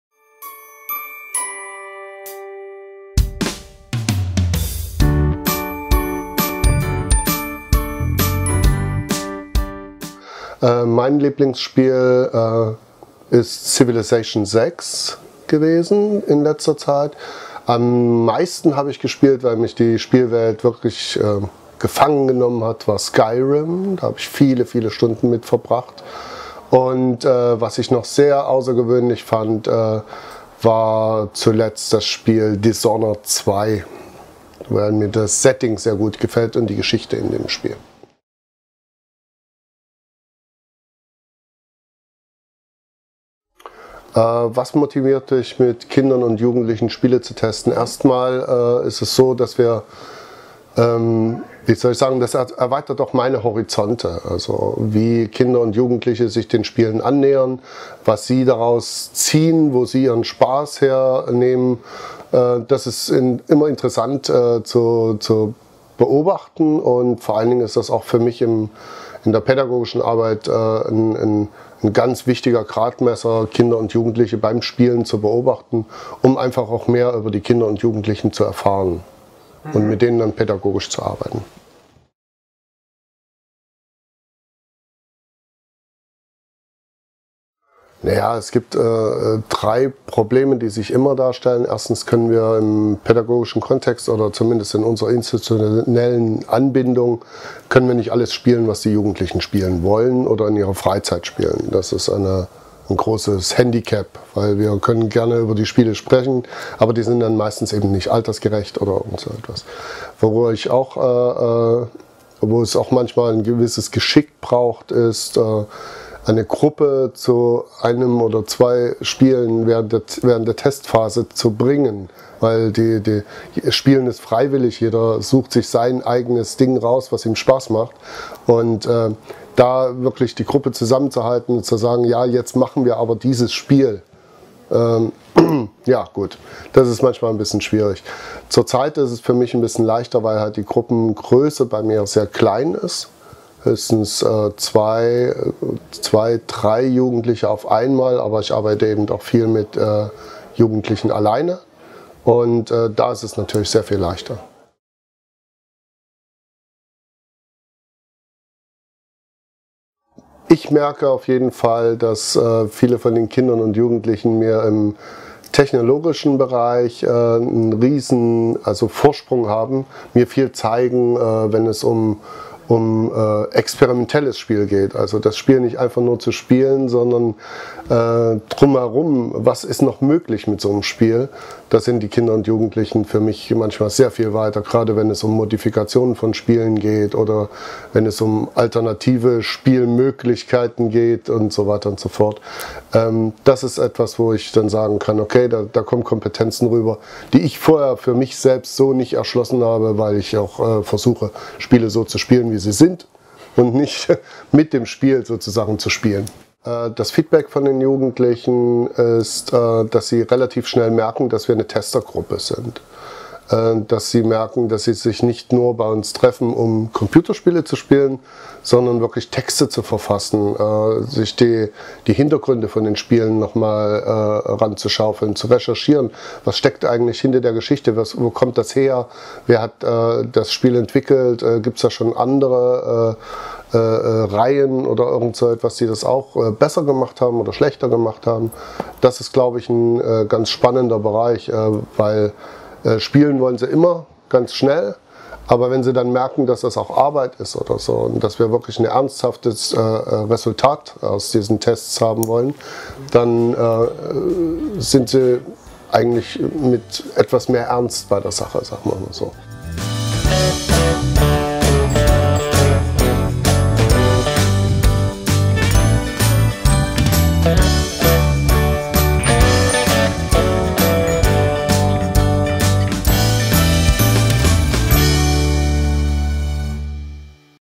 Unsere Gruppenleiter_innen erzählen: Dirk Poerschke | Spieleratgeber NRW
Die Beurteilungen auf der Spieleratgeberseite schreiben sich nicht von alleine und ist auch nicht die einzige Aufgabe von uns. Wir kümmern uns zusammen mit Experten_innen auch um die Kinder- und Jugendarbeit. In dieser Videoreihe stellen sich die Gruppenleiter_innen Fragen aus der Redaktion.

Wissen was gespielt wird! Der pädagogische Ratgeber zu Computer- und Konsolenspielen.